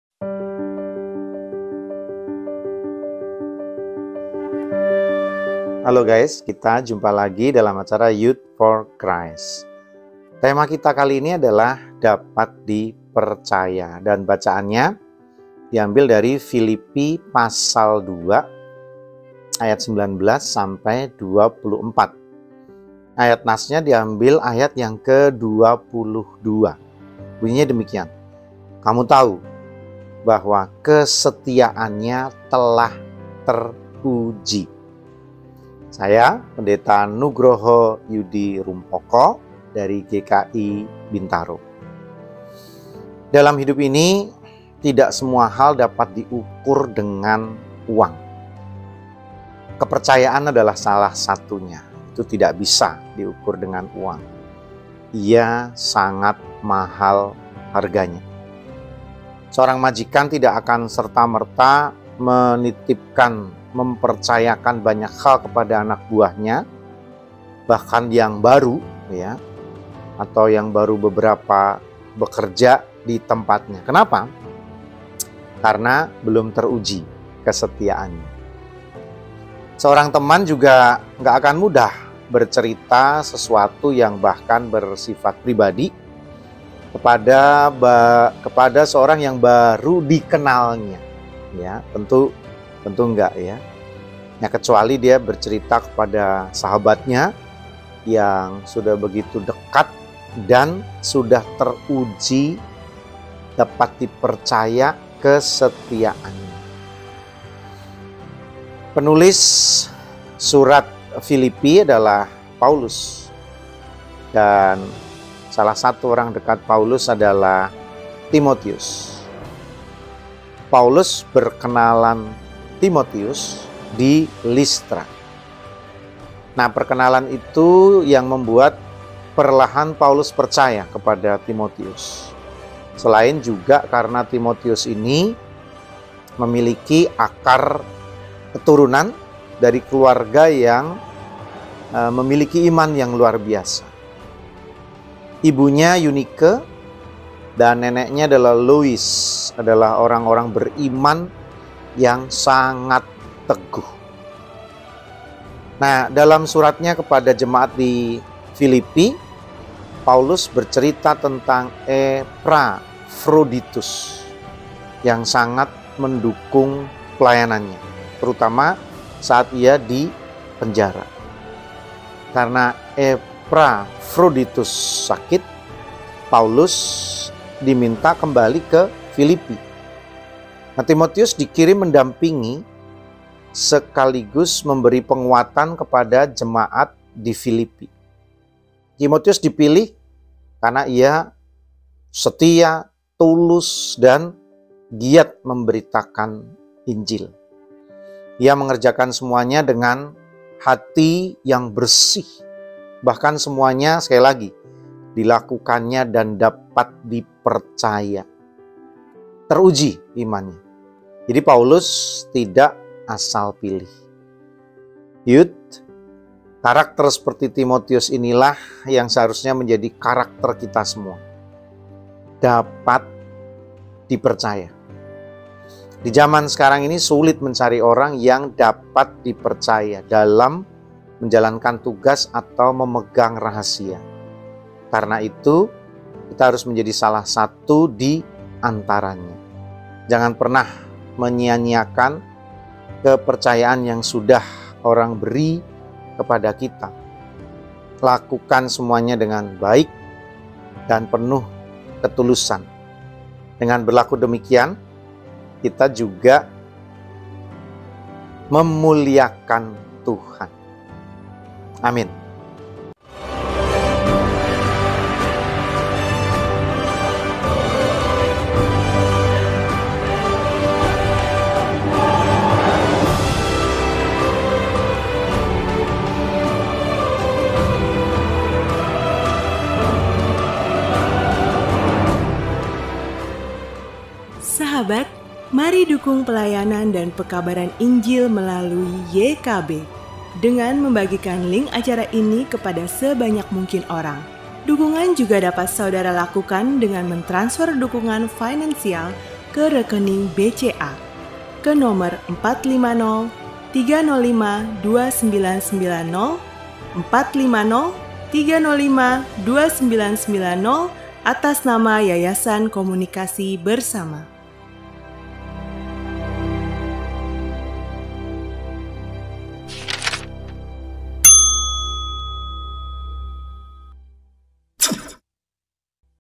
YOUTH FOR CHRIST 388 - DAPAT DIPERCAYA
Renungan Harian Pemuda “Youth For Christ” untuk hari ini, Kamis, 31 Maret 2022
berjudul : DAPAT DIPERCAYA
Didasarkan pada : Filipi 2:19-24

Dibawakan oleh : Pdt. Nugraha Yudi Rumapaka